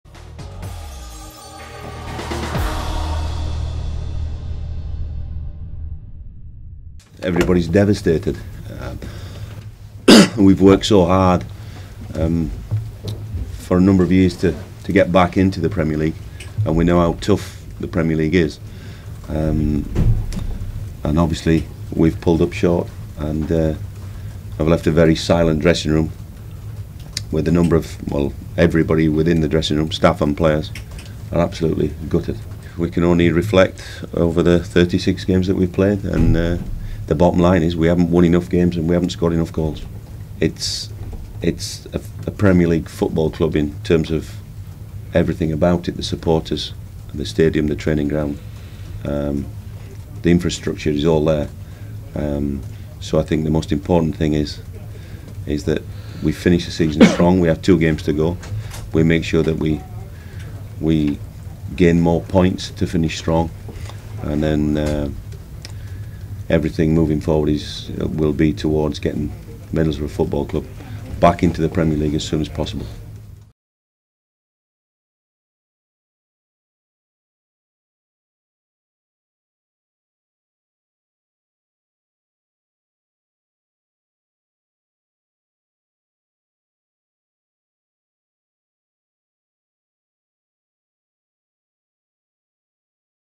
Steve Agnew Reflects On Middlesbrough's Relegation From The Premier League
Despondent Middlesbrough head coach Steve Agnew hopes to finish the season on a high after relegation from the Premier League was confirmed with a 3-0 loss at Chelsea.

Thanks